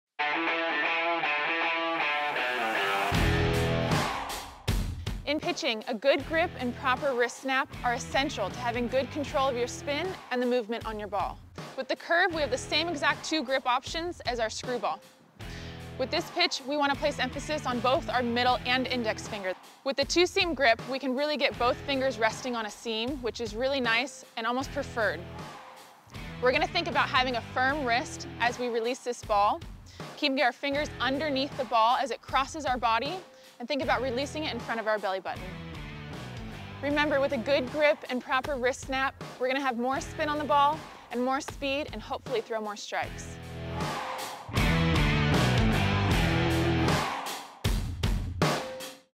How to Grip and Snap a Curveball in Softball
To throw an effective curveball, your grip and snap are key. The curveball has a similar grip to another breaking pitch, the screwball.

Both pitches give you the option of using a four-seam or two-seam grip. With the four-seam grip, you will look for a “C” on the ball’s seams. However, for a curveball, many pitchers prefer to use the two-seam grip as it allows them to have a finger on each seam. When throwing a curveball, emphasis is placed on the middle and index fingers.

For your curveball snap, you will keep your wrist firm and fingers underneath the ball. Try to release the pitch in front of your belly button.

SOCIAL
Become a part of the DICK’S Sporting Goods community: